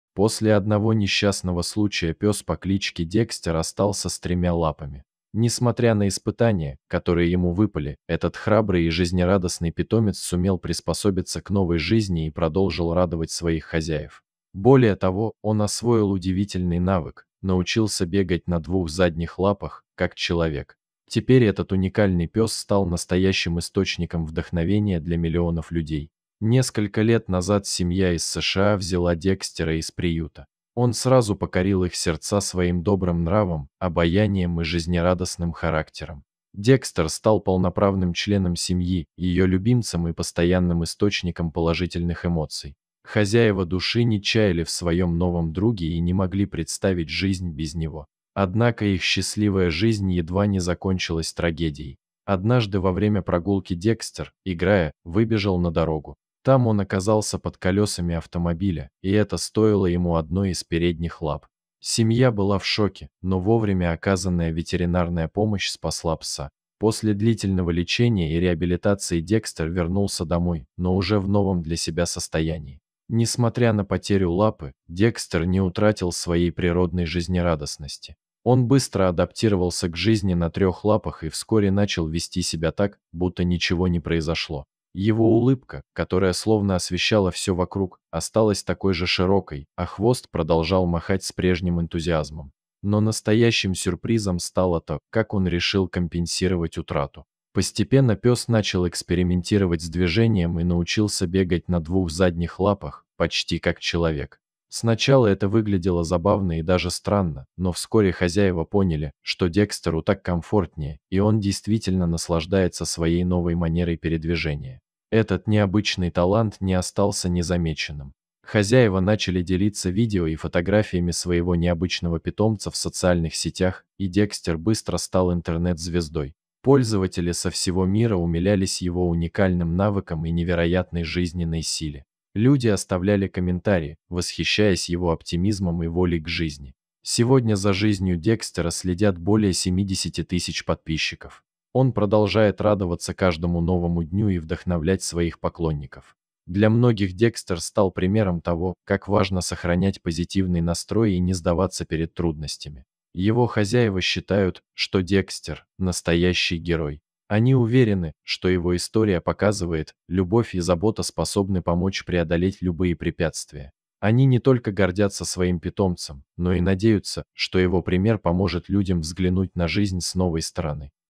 Пёс потерял лапу, после того, как его сбил автомобиль.. Но он начал ходить на двух лапах!
После одного несчастного случая пес по кличке Декстер остался с тремя лапами. Несмотря на испытания, которые ему выпали, этот храбрый и жизнерадостный питомец сумел приспособиться к новой жизни и продолжил радовать своих хозяев. Более того, он освоил удивительный навык – научился бегать на двух задних лапах, как человек. Теперь этот уникальный пес стал настоящим источником вдохновения для миллионов людей.
Несколько лет назад семья из США взяла Декстера из приюта. Он сразу покорил их сердца своим добрым нравом, обаянием и жизнерадостным характером. Декстер стал полноправным членом семьи, ее любимцем и постоянным источником положительных эмоций. Хозяева души не чаяли в своем новом друге и не могли представить жизнь без него.